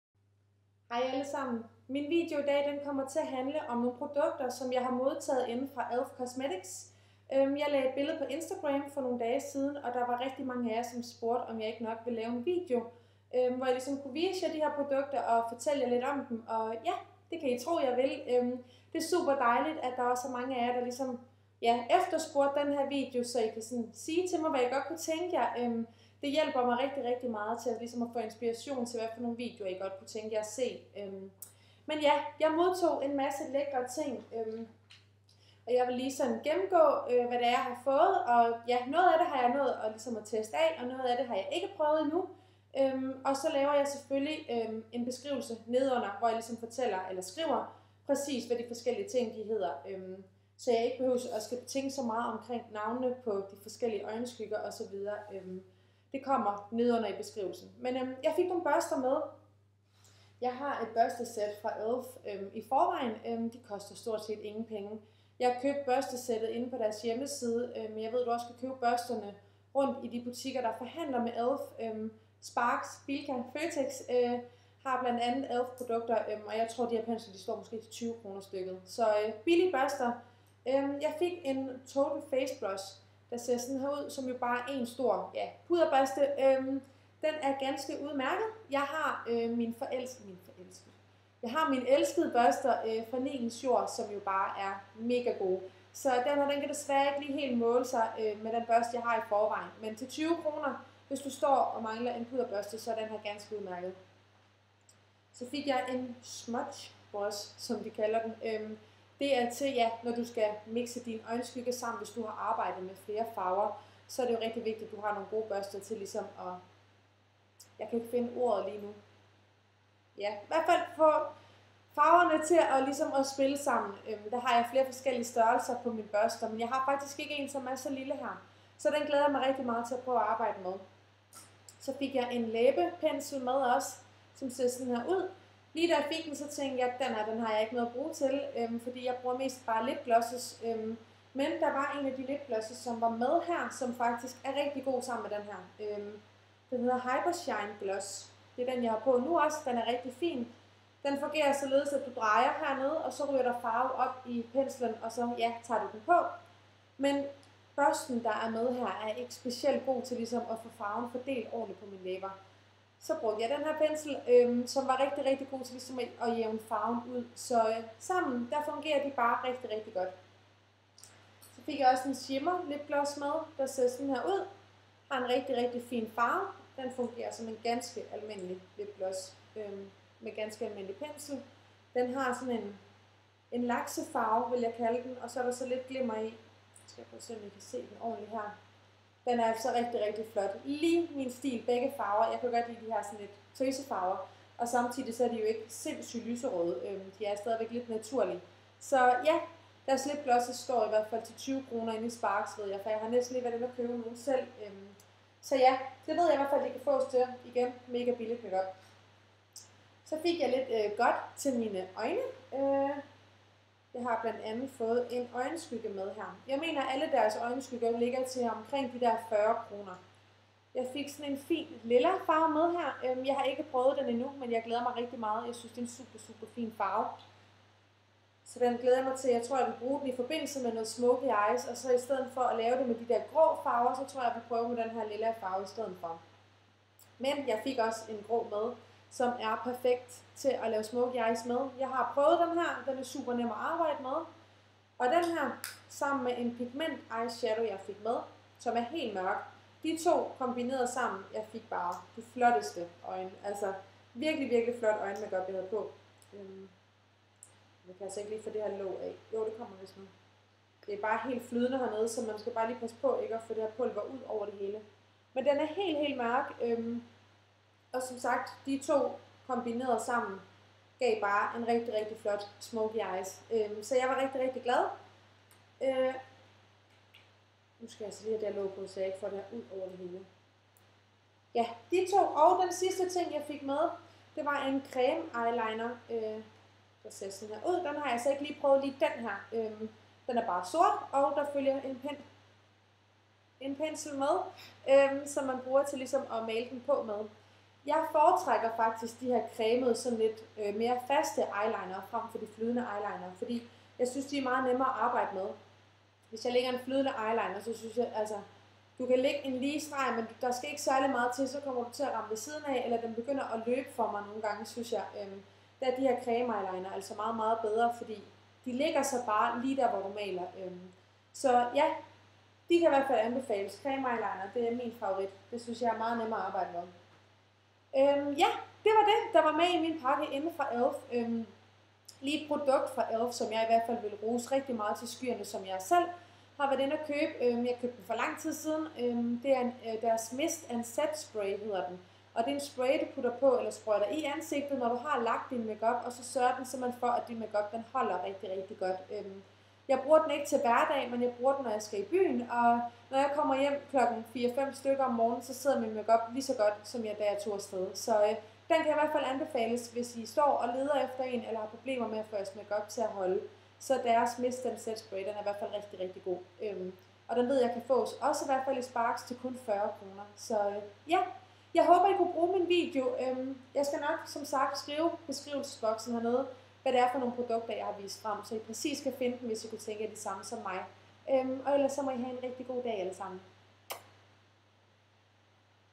Mine nye ELF produkter!
1. Total Face Brush

2. Smudge Brush

3. Lip Defining Brush

4. Hypershine Gloss - Farven: Flirt

5. Shimmer Lip Gloss - Farven: Inspire

6. Long-lasting Lustrous Eyeshadow - Tror min er den farve der hedder Festivity

7. Baked Eyeshadow - Farven: Dusk

8. Pigment Eyeshadow -Farven: Naughty Noire

9. Cream Eyeliner - Farven: Sort